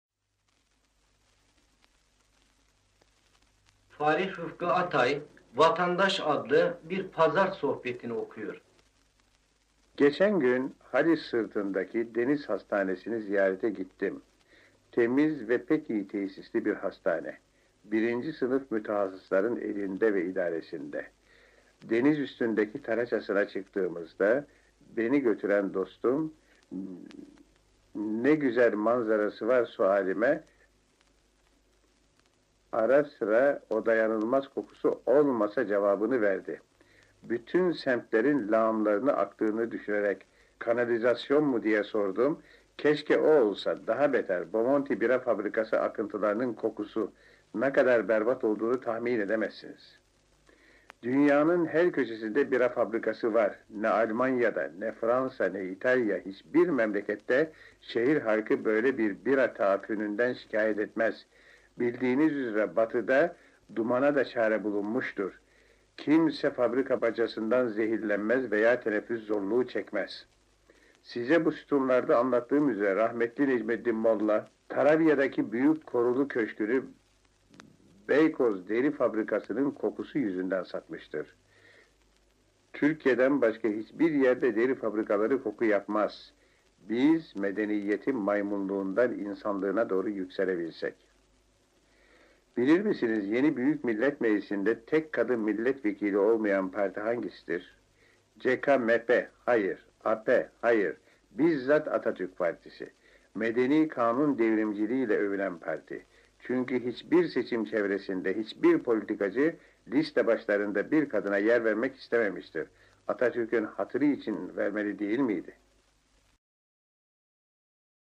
B7 FALİH RIFKI ATAY - BİR PAZAR SOHBETİNDEN VATANDAŞ - Kendi Sesinden - INDIANA UNIVERSITY RECORDS
Yazar Prof. İLHAN BAŞGÖZ önderliğinde, INDIANA UNIVESITY`de Türk şair ve yazarlarının kendi seslerinden kayıtları, albüm şeklinde düzenlenmiştir. Kayıtlar Fikret Otyam`ın muhabirlik döneminde 1963 yılında gerçekleşmiştir.